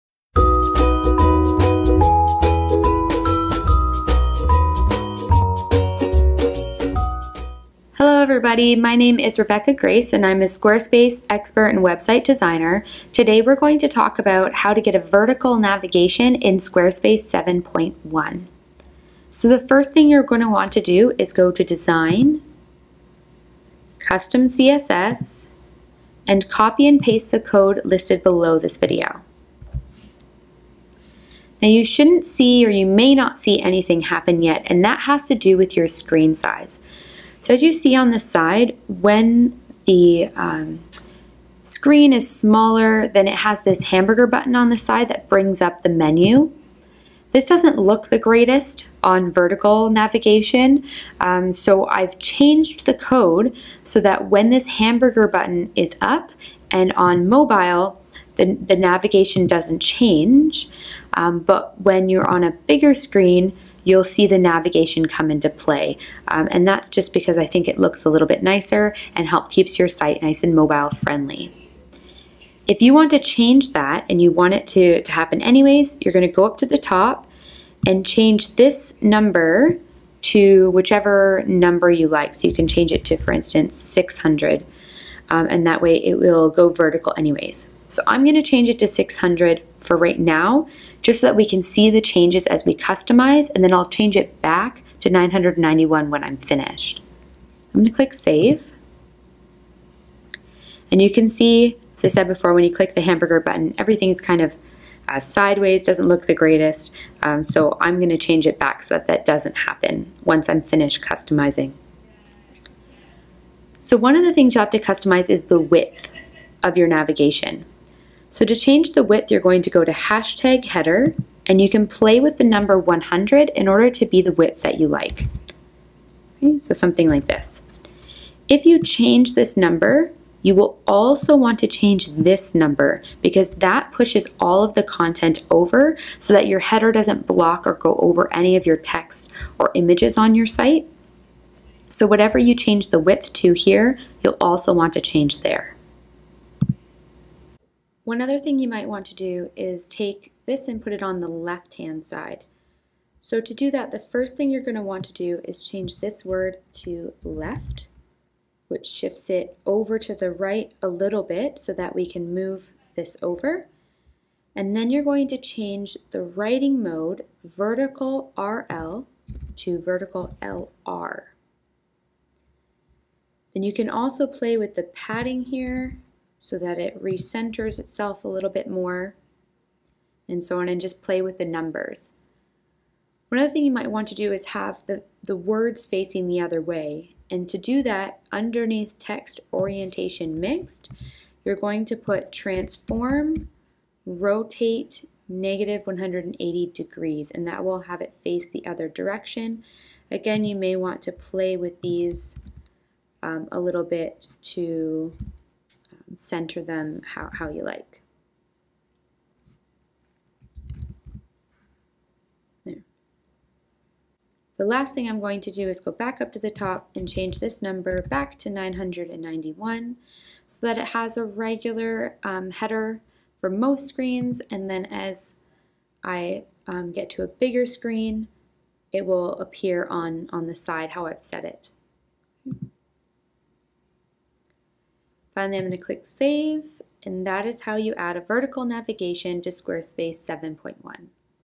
Vertical Navigation | Squarespace 7.1 (CHECK OUT UPDATE BELOW)
**UPDATES**
A lot has changed since this video was recorded! If this code is no longer working for you check out my Vertical Navigation Plugin! Unlike Blog Videos, this plugin is continually updated to align with any Squarespace Updates!!

In this video, I show you how to create a Vertical Navigation in Squarespace 7.1.

For more tips and tricks on customizing your Squarespace Website join my Squarespace Tips and Tricks Community